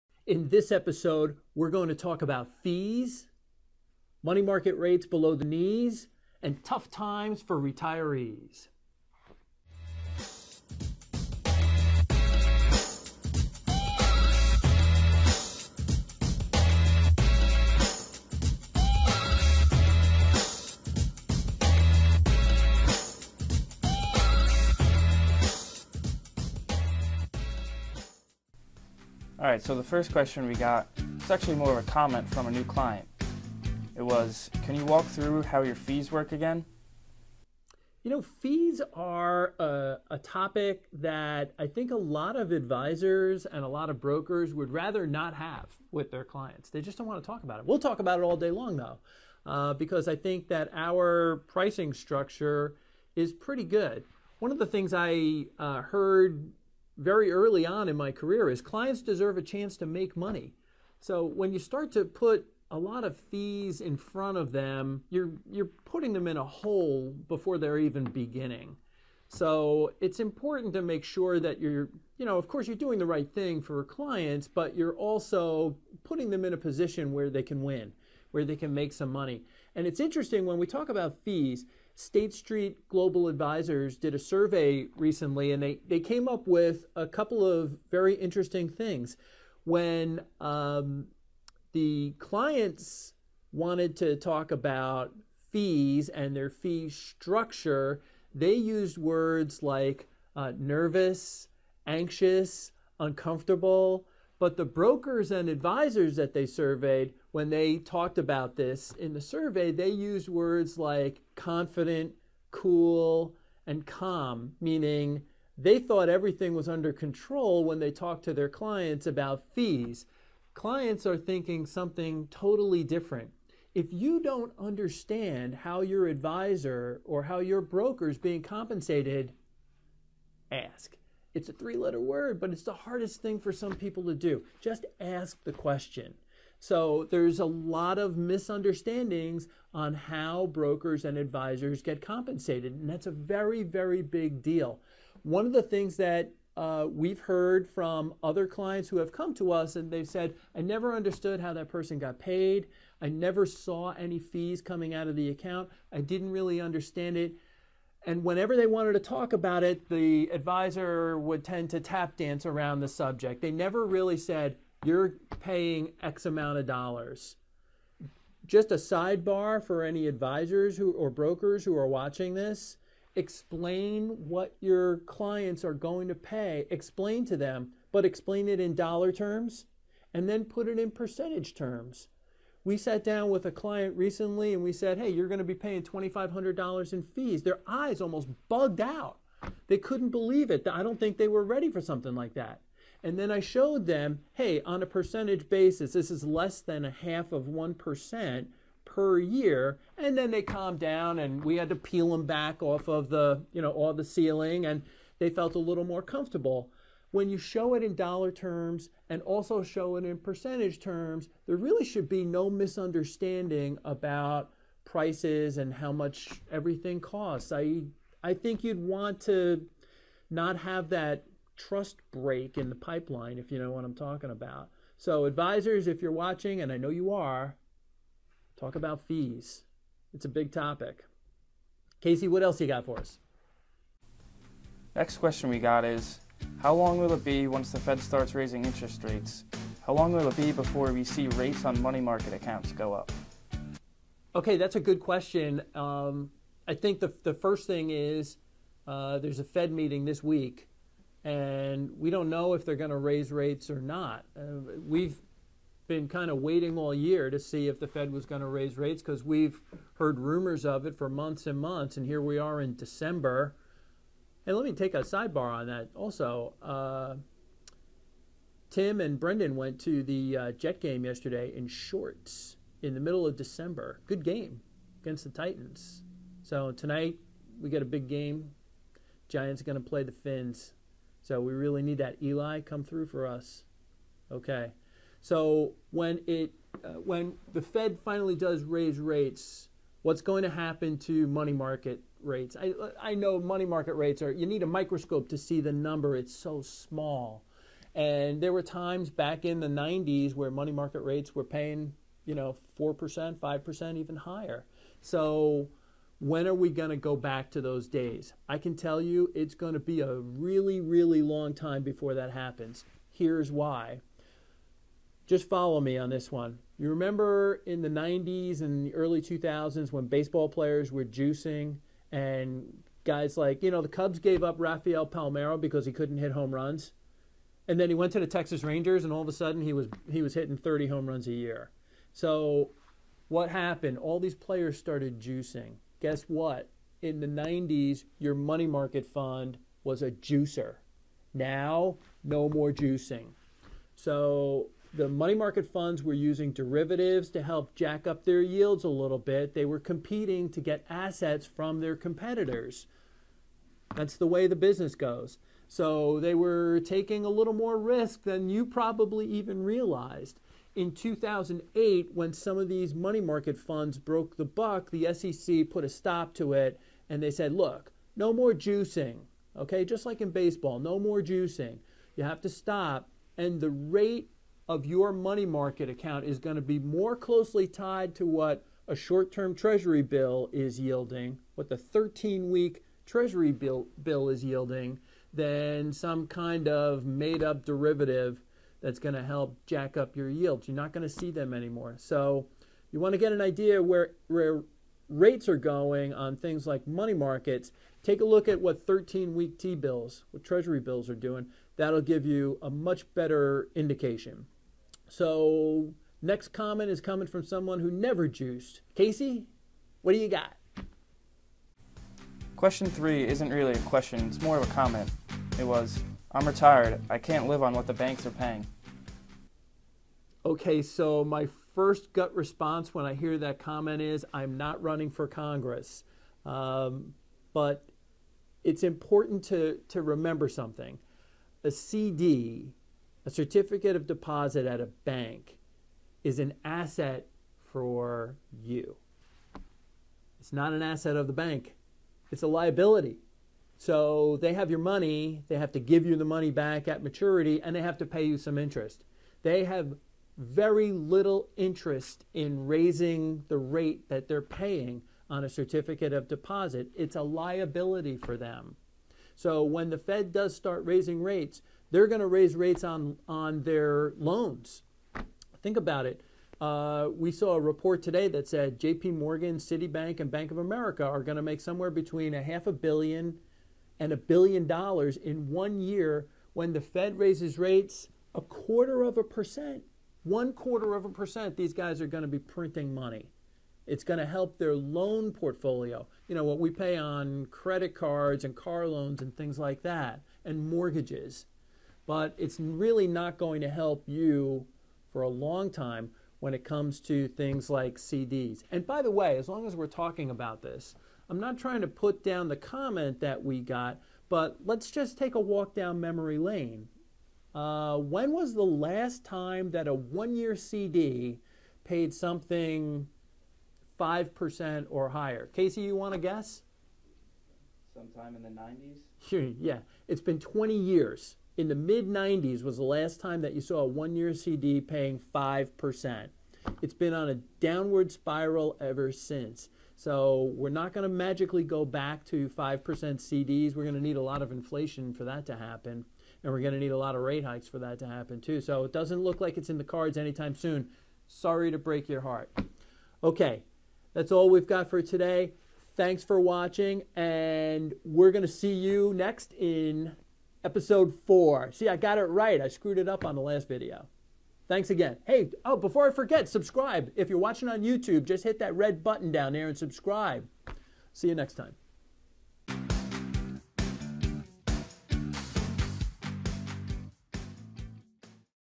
Mullooly Asset Show Episode 3: Talking Fees, Rates Below the Knees & Tough Times for Retirees
Welcome to the Mullooly Asset Show

Timestamps:
0:27 - "Uncomfortable" discussion on Advisor Fees
3:53 - Rates on Money Market funds?
6: 50 - "Can't Live On What Banks Are Paying"